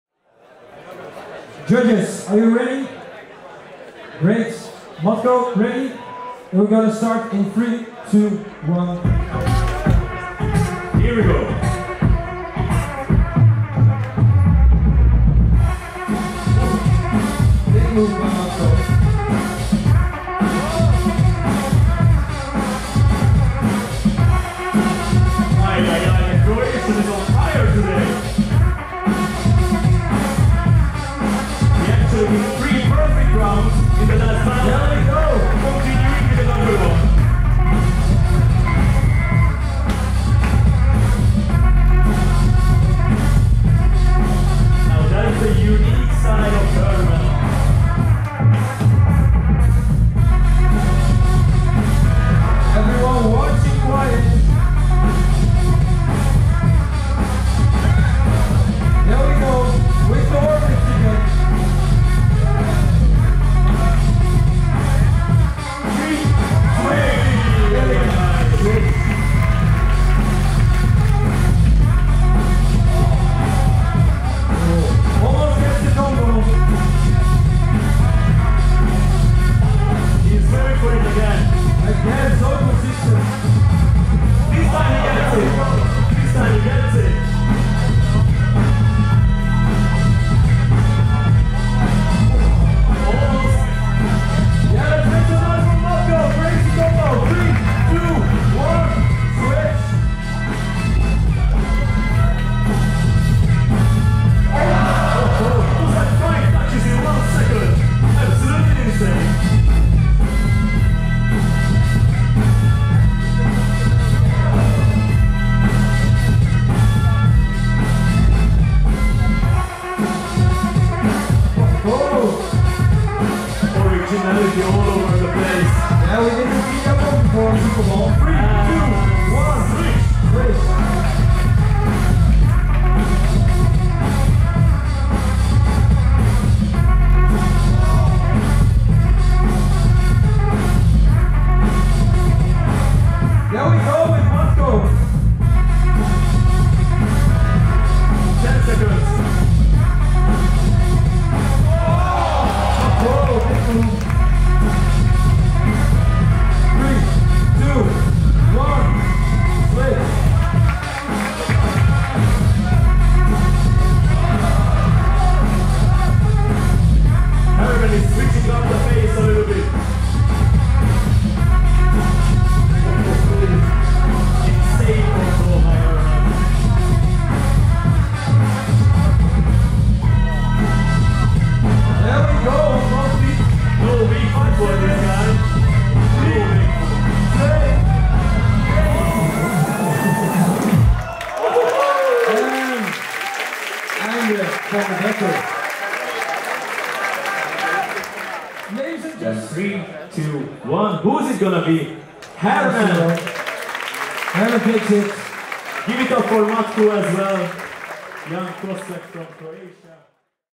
Matko vs Hermen | Top 32 | Super Ball 2024
Matko (HRV) vs Hermen (NED) | Top 32 | Super Ball World Freestyle Football Championships 2024

Winner: Hermen (5-0)

Freestylers: @hermen.voerknecht, @matkofreestyle
Footage: @jako17b